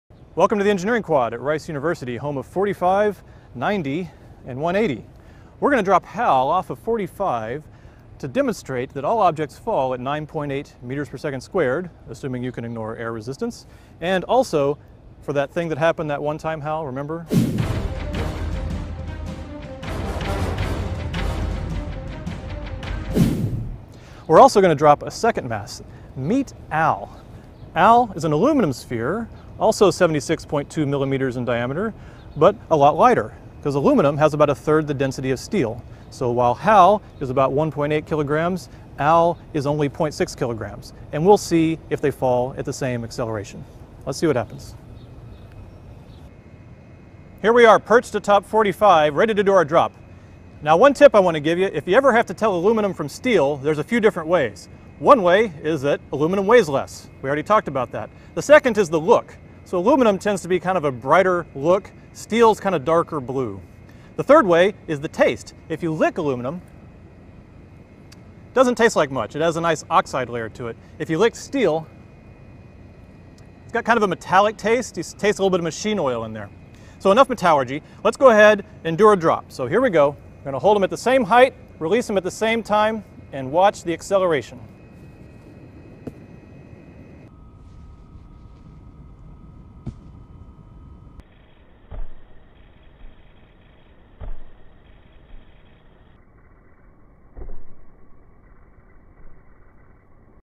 DEMO: Constant Acceleration
Here we demonstrate that the acceleration due to gravity near the surface of the earth is constant for different masses.  The two spheres are identical in size but one (steel) is three times the mass of the other (aluminum).  They are dropped from the top of a large granite statue at Rice.  They hit at *exactly* the same time, which is a testament to Galileo's genius and to Hafner's ability to precisely drop the ball.